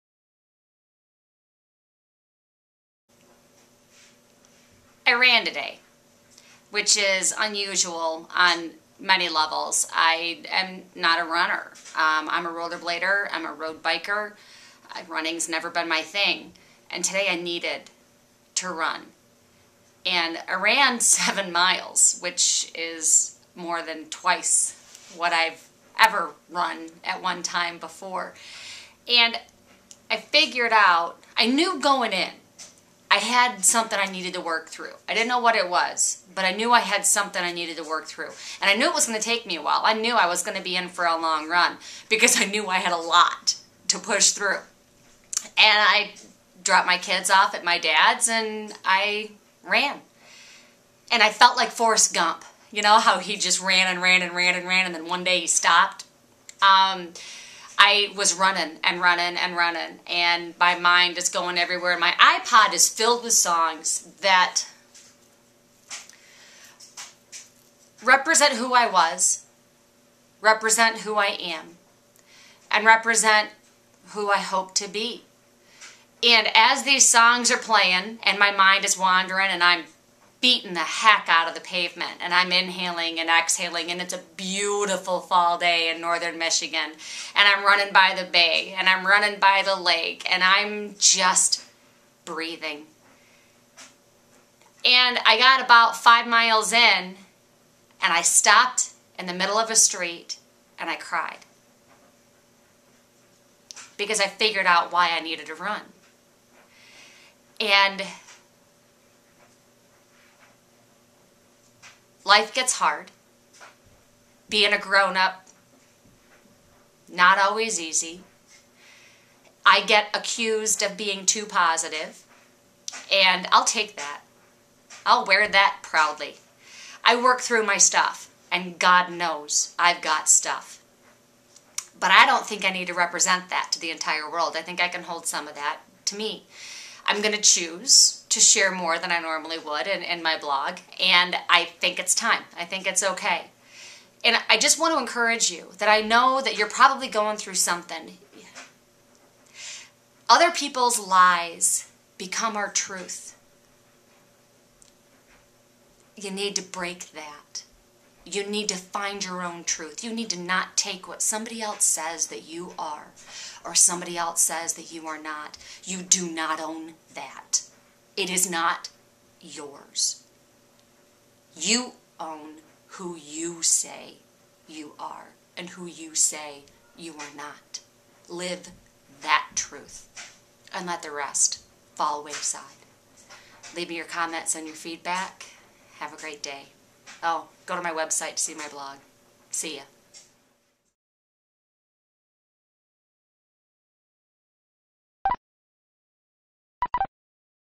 Run, Forrest! RUN!!! Truth & Lies Stacey Hanna
I'm not a runner. So when I crank out 7 miles because the voices in my head are loud and need an outlet--you gotta know that I had something big to work though. I use my workouts to work through crap. Emotions. Feelings. My past. My future. My life. Me.

Other people's lies become our truth and that's not okay. Other people have opinions and thoughts of who you are--but what YOU think of you is WAY more important.

Hit up my blog for, as Paul Harvey used to say, the rest of the story.

Stacey Hanna